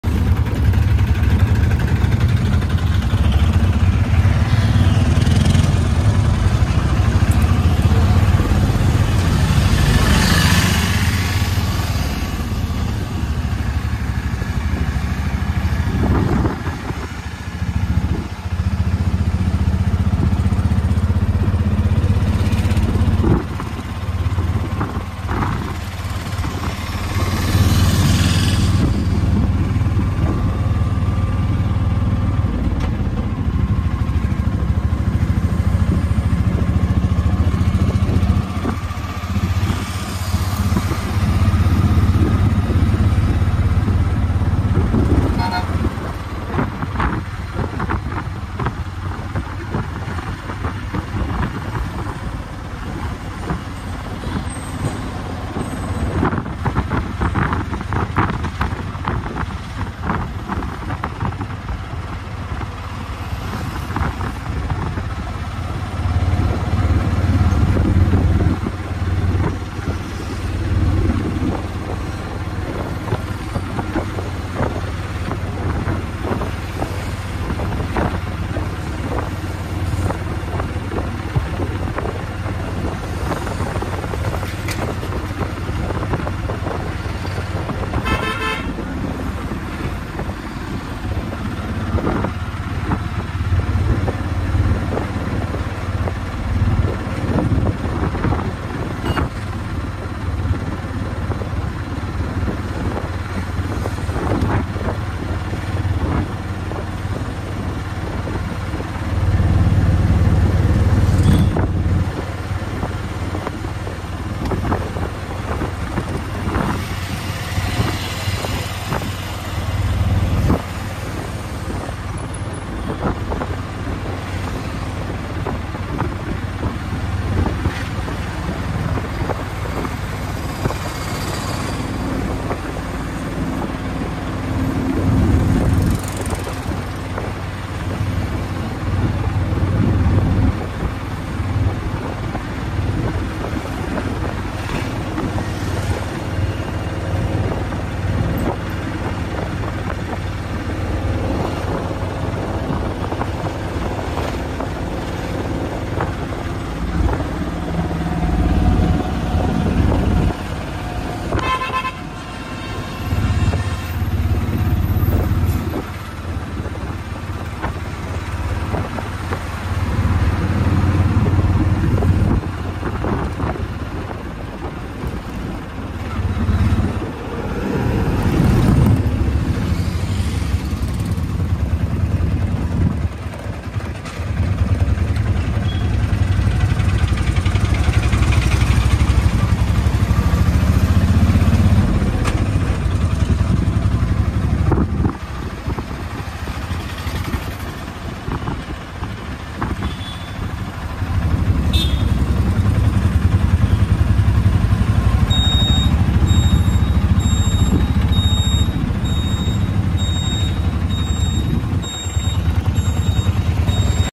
Tuk Tuk Crazy Drive in Highway
Tuk tuk travel in a highway road